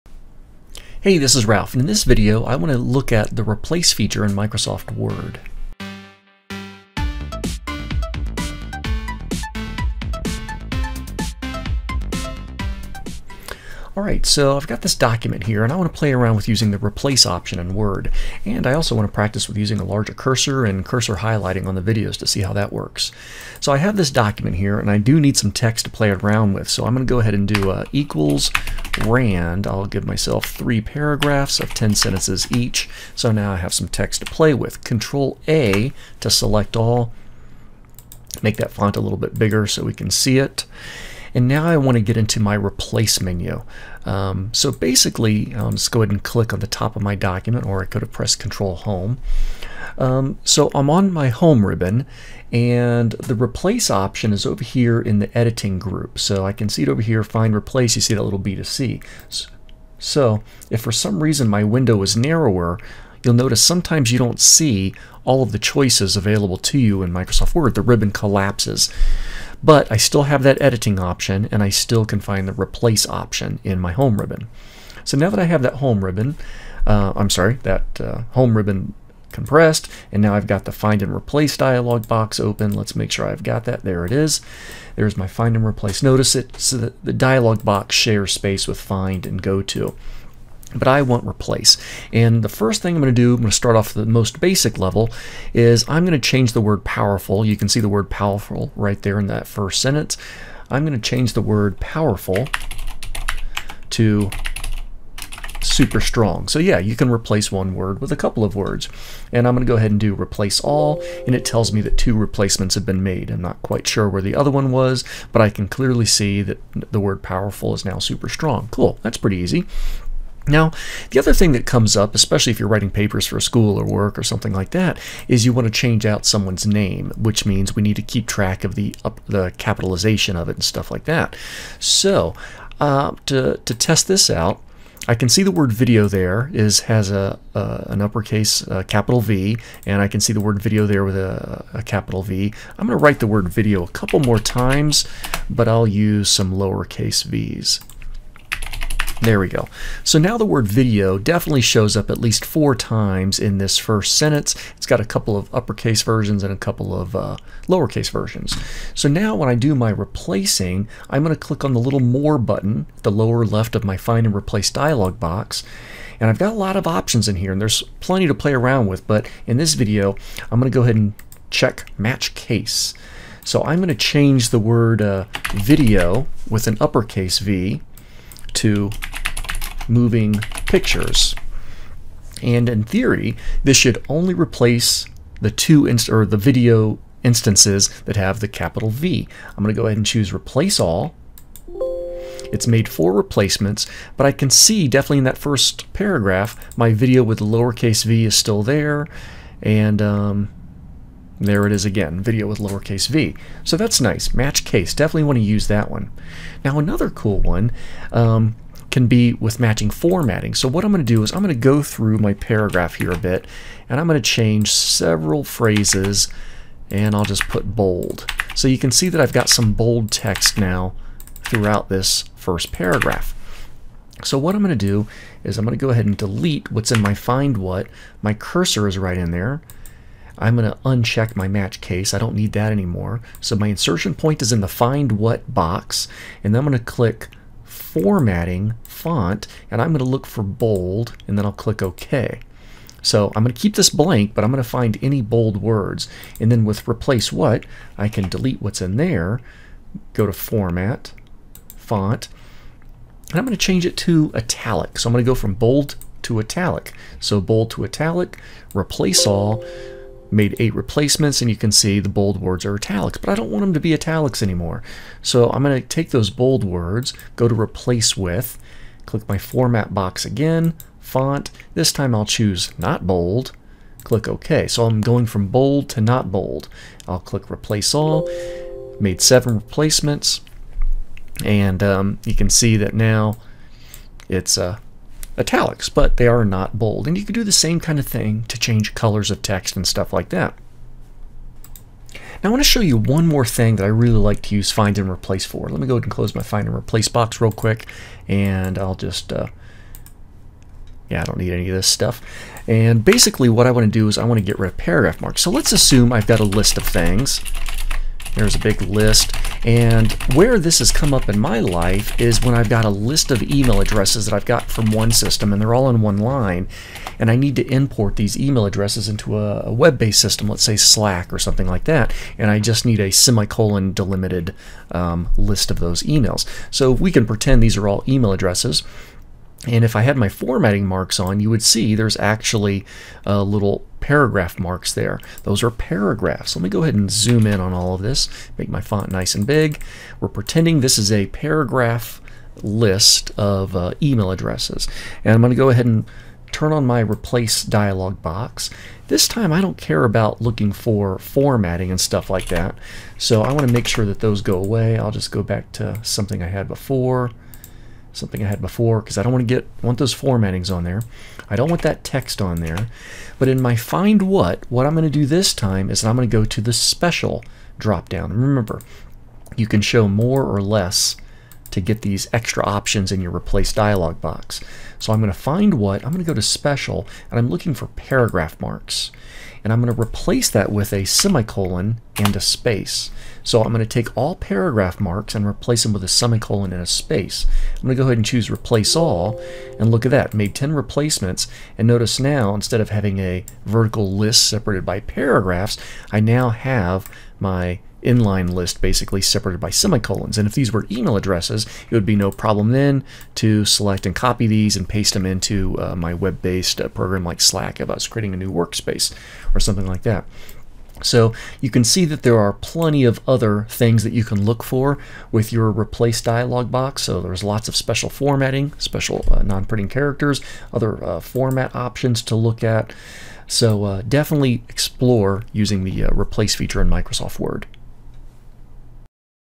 Find and Replace Text, Formats, and Paragraph Marks in Word
Use the Find and Replace feature in Microsoft Word to replace...
one word with another word
a person's name with another name
bold words with italicized words
paragraph marks to semi-colons